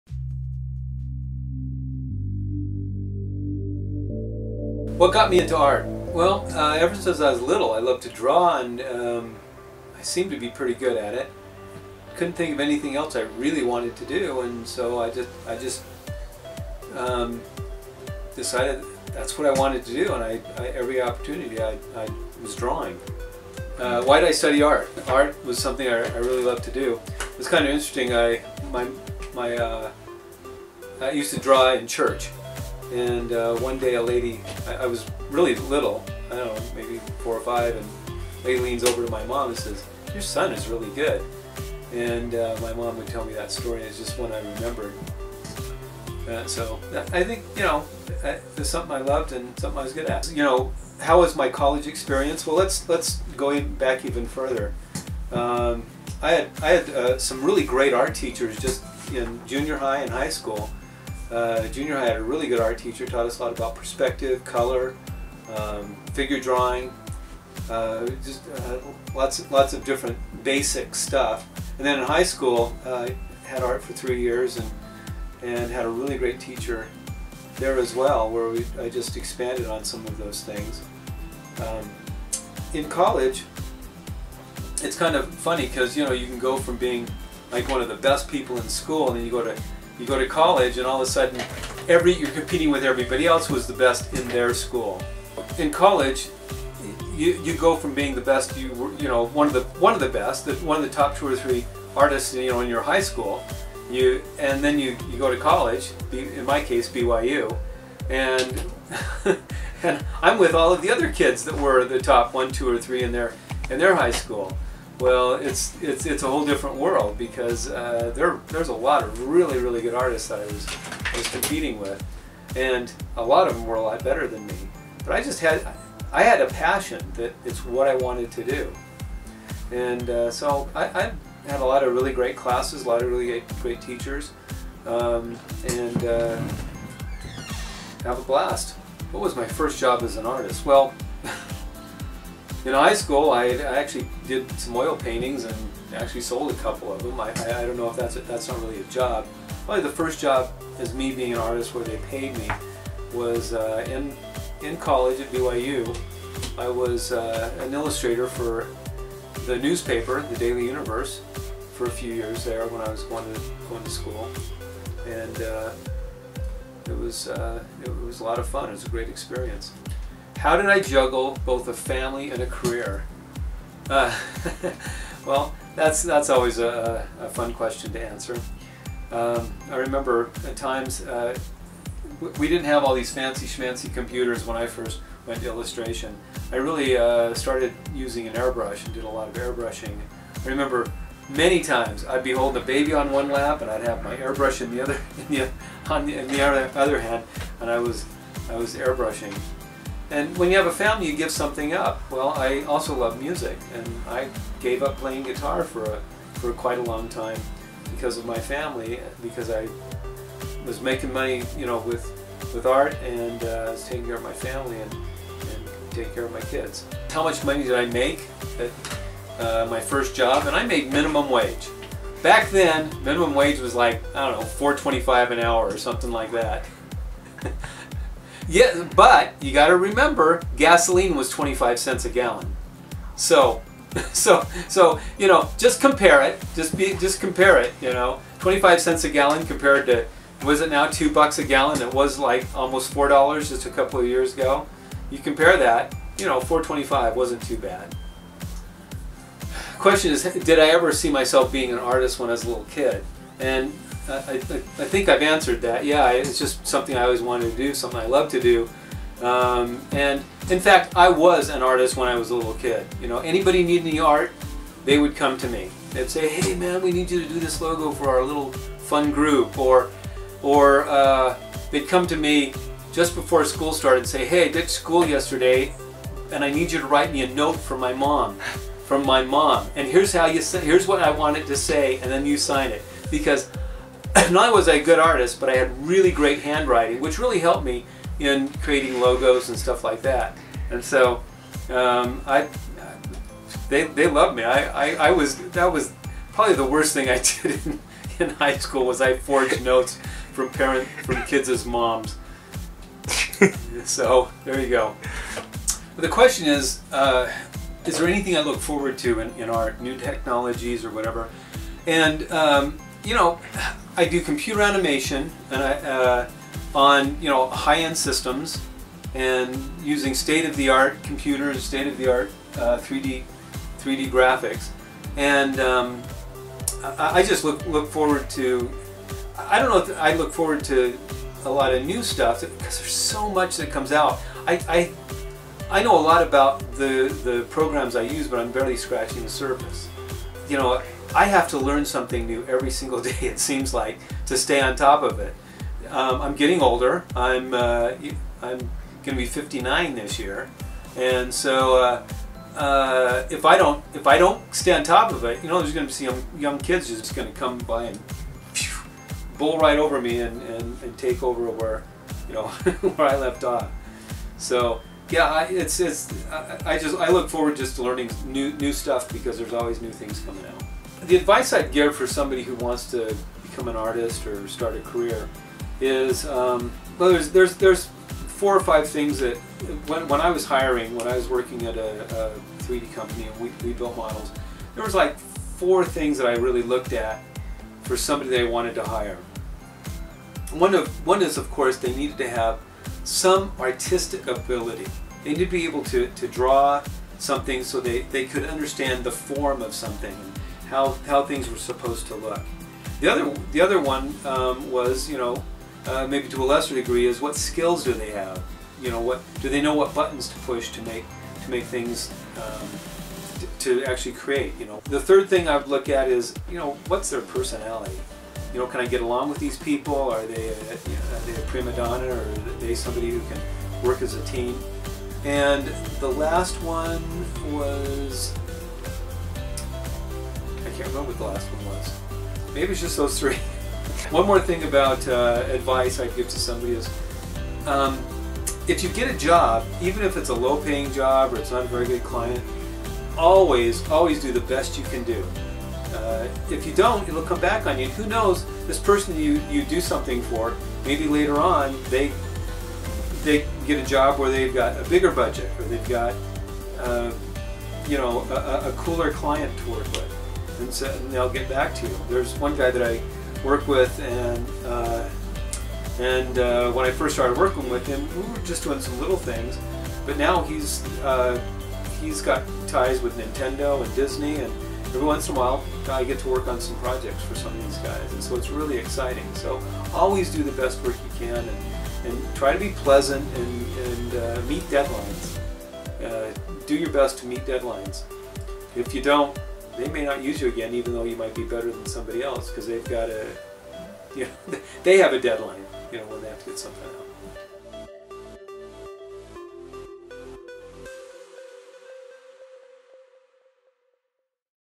Steven Keele: An Artist
A look at Steven Keele's successful art career.
He talks about his beginnings and shares his wisdom.